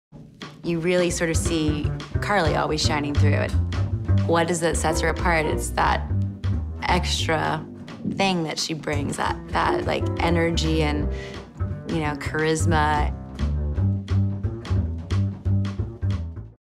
Christy Turlington Burns on Karlie Kloss: Extraordinaries | Cole Haan
Christy Turlington Burns shares how Karlie Kloss inspires her.

Shot & filmed by Cass Bird